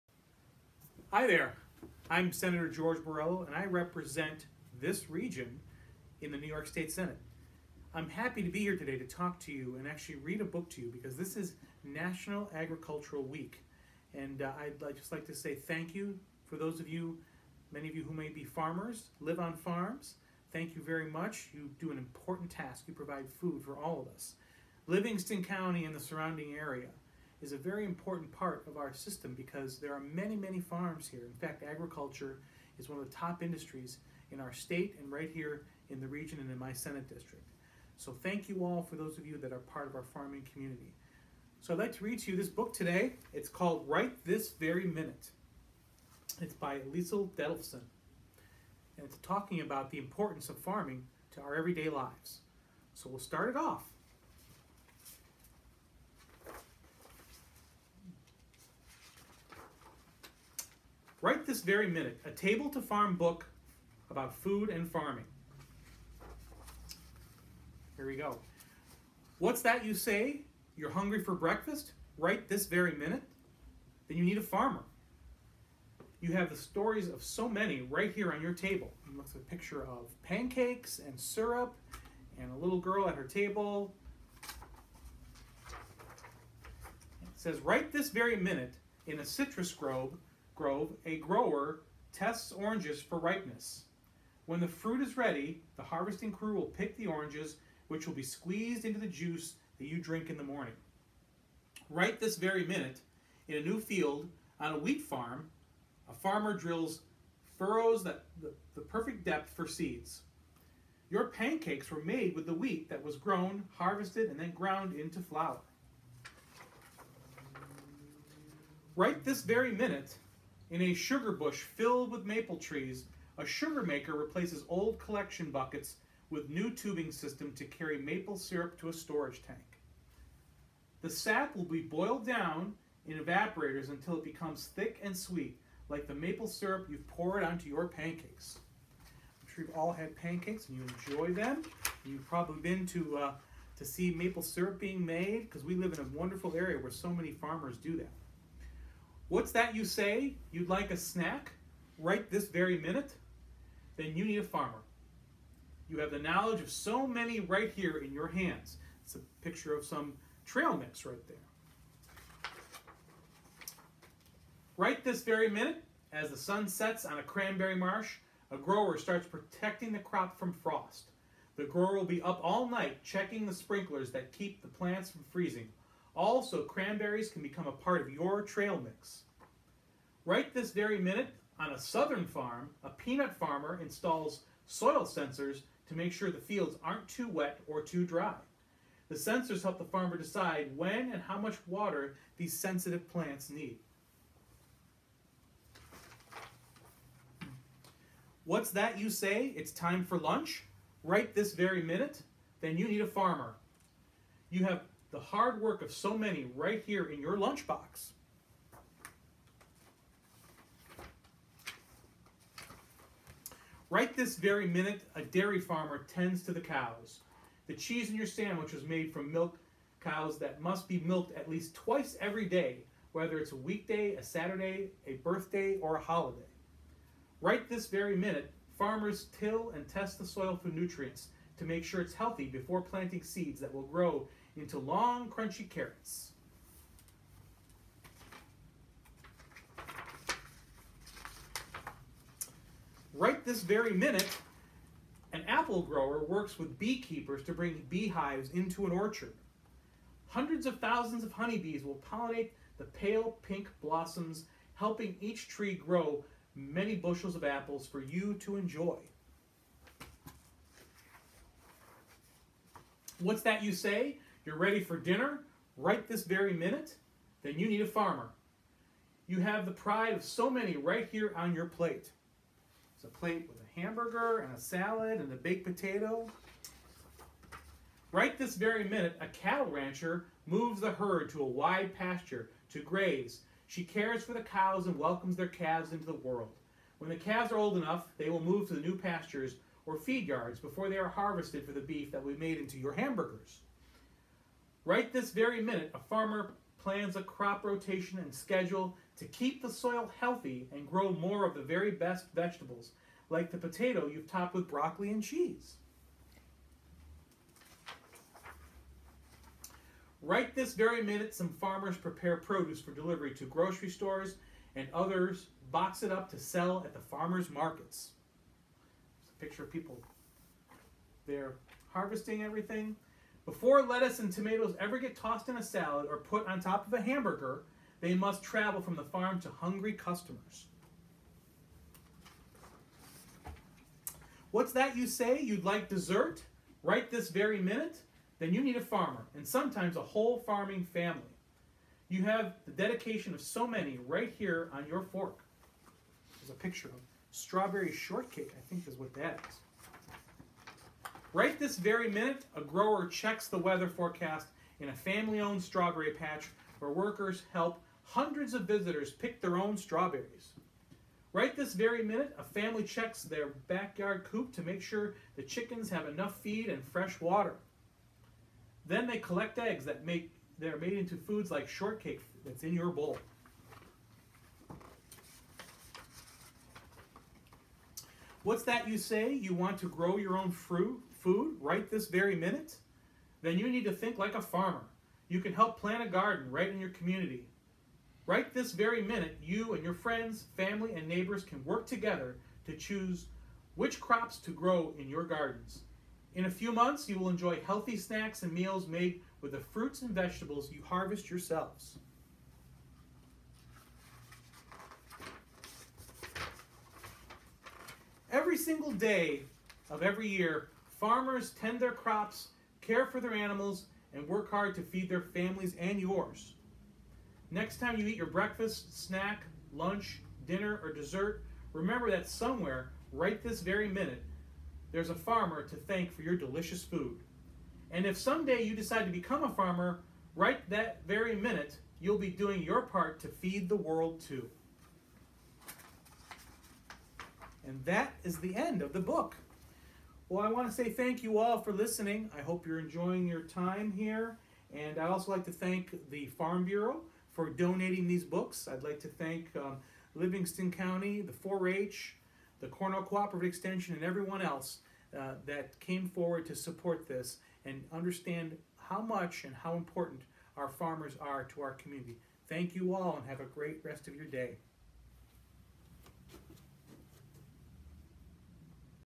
"Right This Very Minute" 2020 Ag Literacy reading by NYS Senator George Borrello
New York Senator Borrello read "Right This Very Minute" for you!  This book is the 2020 New York Agricultural Literacy book.  Our Agriculture in the Classroom program worked with Senator Borello and his wonderful staff to bring this book to you. Enjoy!

This recording was done with permission from Feeding the Minds Press, the American Farm Bureau Foundation.  We appreciate their support and great work to help spread the good work of our nation's farms and supply chain.